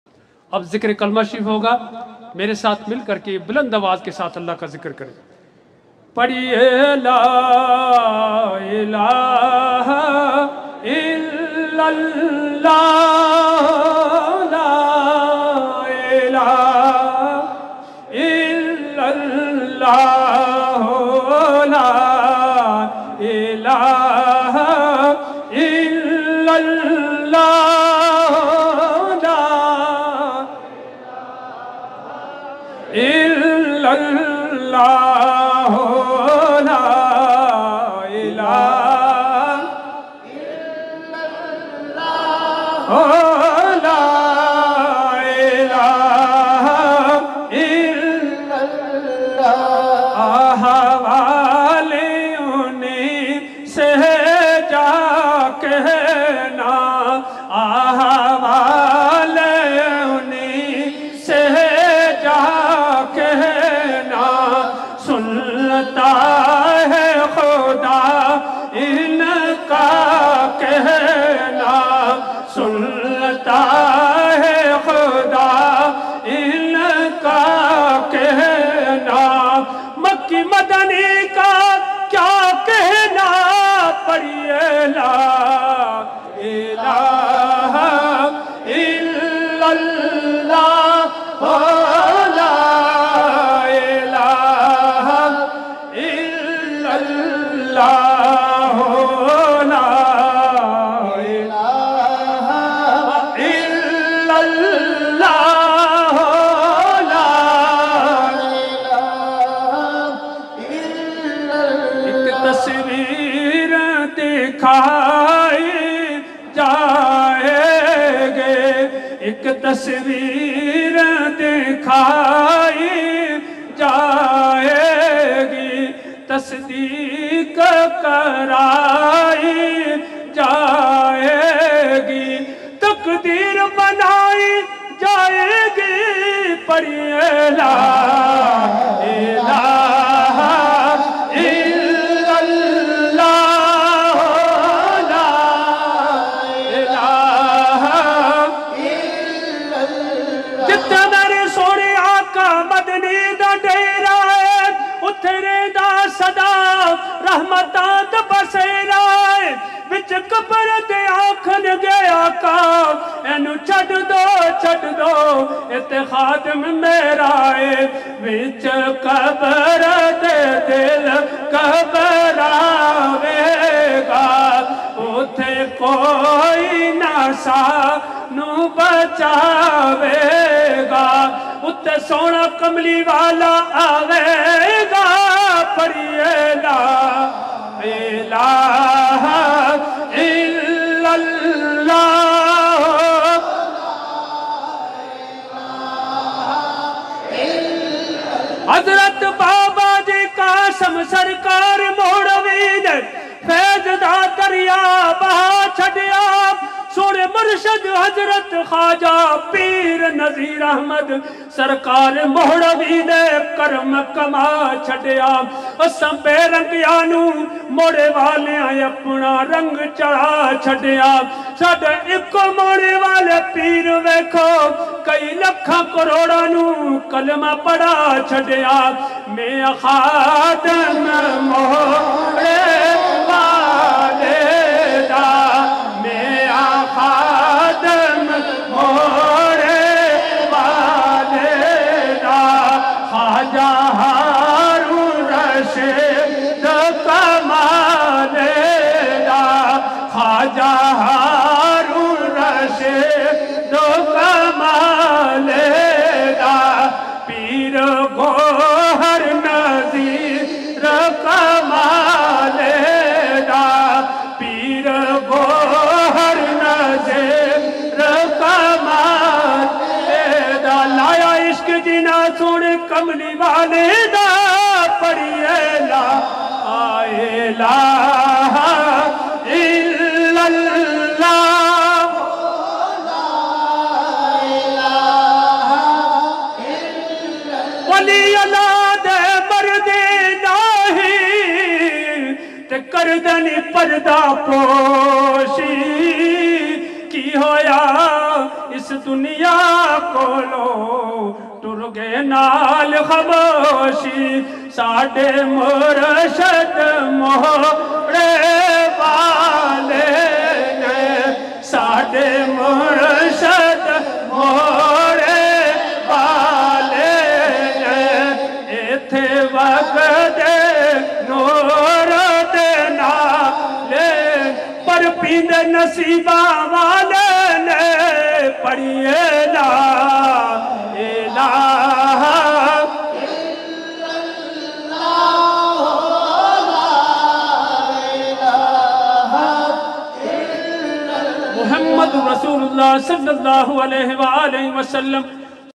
Zikir Kalma Sharif June Urs 2023 at Mohra Sharif.
Mohra Sharif mein salana urs k moqa par mehfil mein zikir kalma sharif qari Siddique sahib ki awaz mein pesh hei.